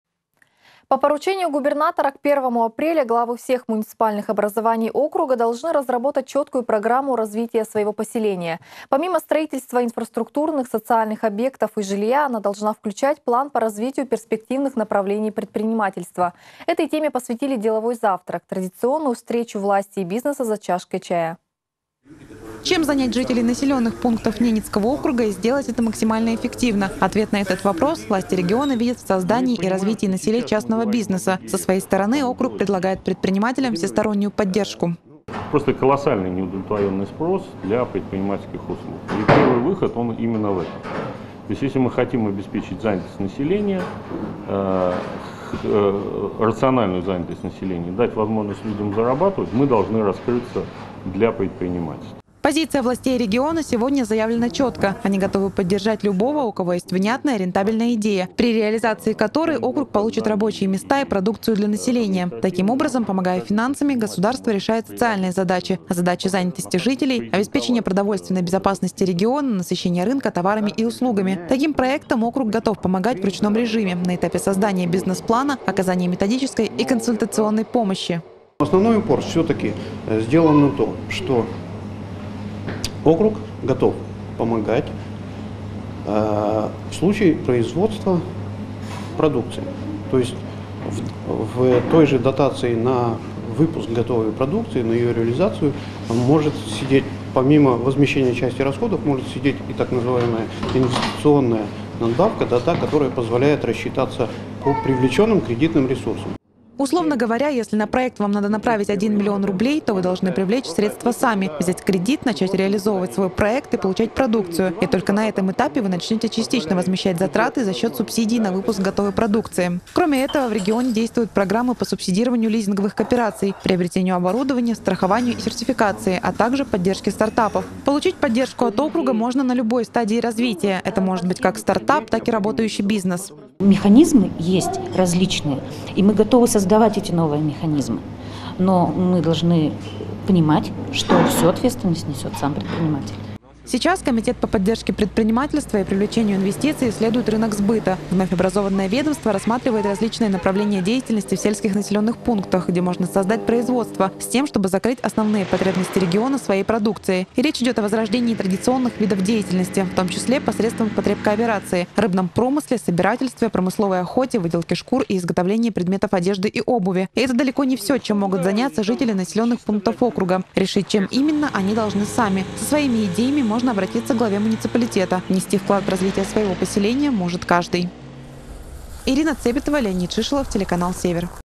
Бизнес-планы появятся у каждого населенного пункта НАО
По поручению губернатора к 1 апреля главы муниципальных образований Ненецкого округа должны разработать четкую программу развития своего поселения. Помимо планов по строительству инфраструктуры, социальных объектов и жилья она должна включать проект по развитию перспективных направлений предпринимательской деятельности.